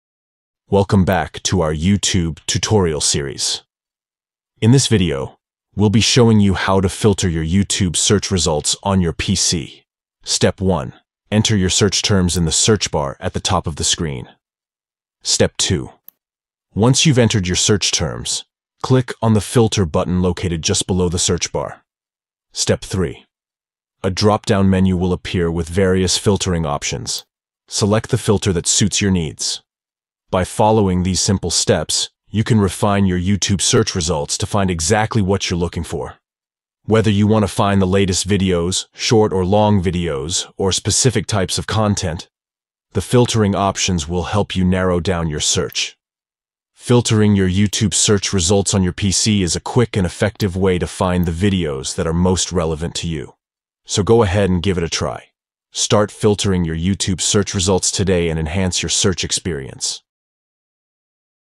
How to Filter YouTube Search Results on PC
Are you tired of sifting through endless YouTube search results? Look no further! In this video, we'll guide you through the simple process of filtering your YouTube search results on your PC.

Step 1: Enter your search terms.
Step 2: Click Filter.
Step 3: Select the Filter you want.

By following these steps, you'll be able to refine your search and find exactly what you're looking for. Whether you want to sort by upload date, duration, or specific content types, filtering your YouTube search results on your PC will save you time and frustration.

Join us in this tutorial as we show you how to take advantage of this powerful feature. Get ready to streamline your YouTube search experience like never before.